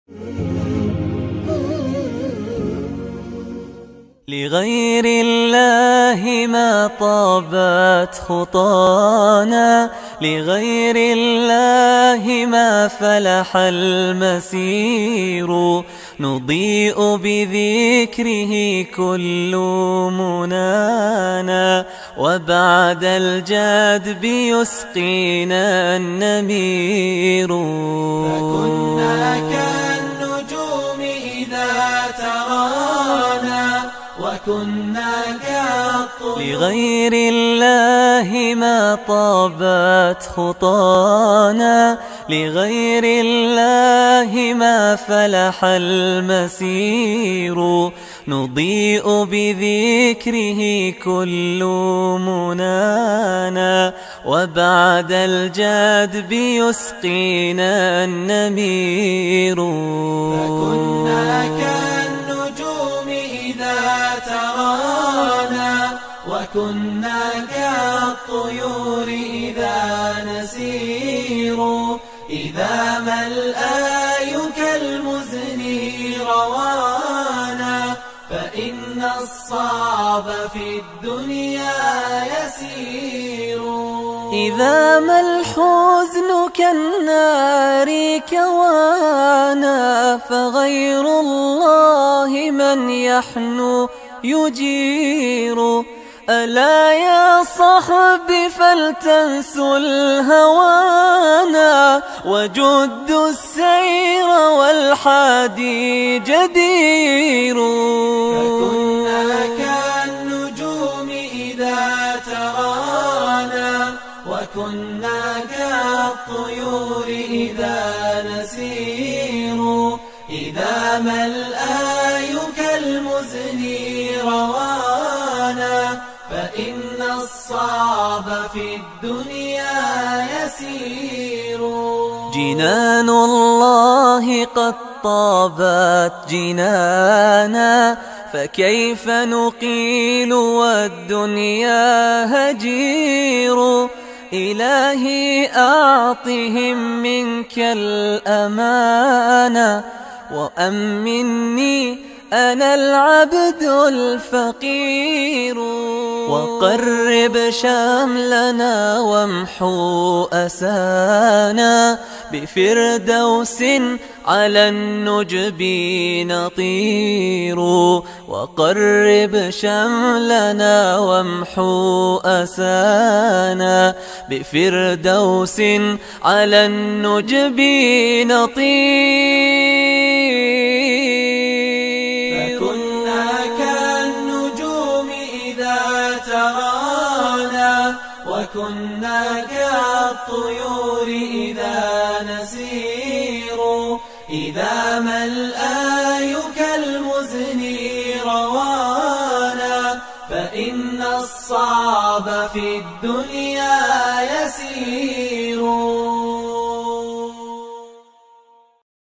Deep Blue Full Coverage Hijab | Hijab with Niqab | Layer Hijab Tutorial 2021 | Niqab Vogue
Assalamualaikum dear sisters 💙💙💙
Hope you all have safe... I'm here with another simple niqab & tutorial. I hope it will be  for you all.

I hope that you will love and collect your best from all video